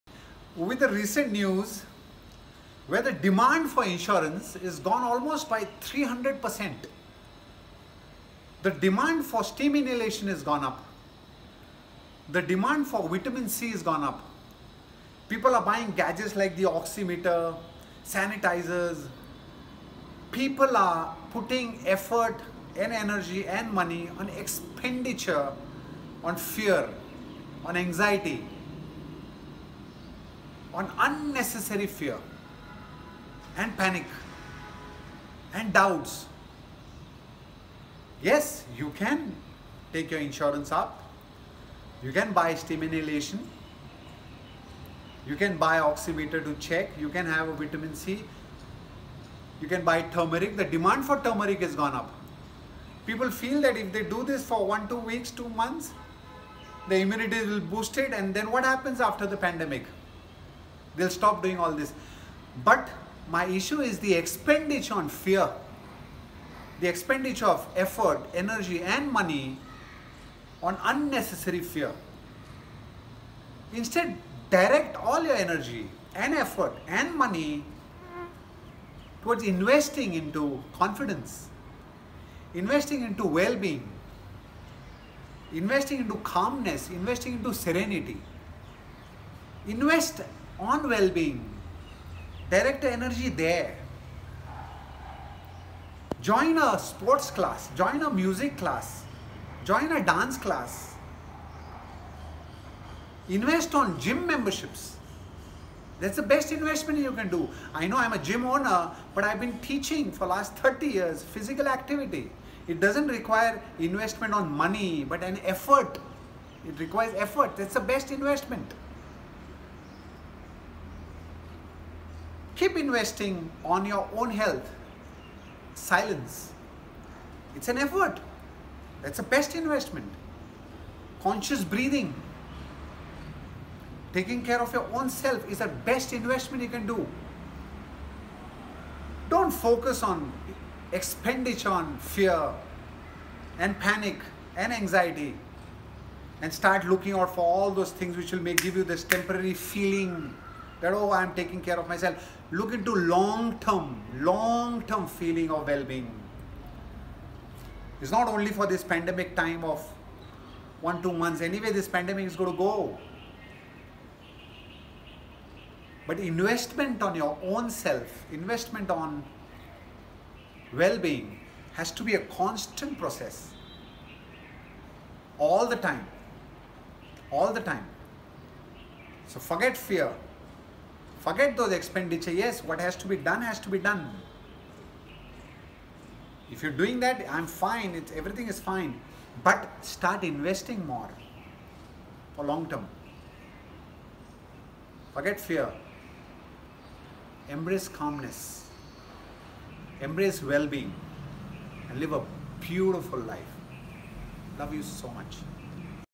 Invest on well being. By Abhimanyu Sable. Founder. ABS FITNESS.
People are spending on fear, anxiety, panic doubt, suspicion.
Demand for
Insurance
Mediclaim
Gadgets- steam inhalers, oximeter.
Home made herbal drinks. Turmeric water.
Supplements of vitamin c and immunity boosters is increasing. Only due to fear.

Invest on calmness
Invest in confidence.
Invest in serenity.
Invest in well being.

Soothing music. Slow and controlled breathing exercises.

Exercise. Stay away from negativity. Use positive vocabulary.

Connect with friend, communicate with loved ones on daily basis.

Pamper yourself. Flow with life.
Accept what is is and what not is not.

Invest on well being.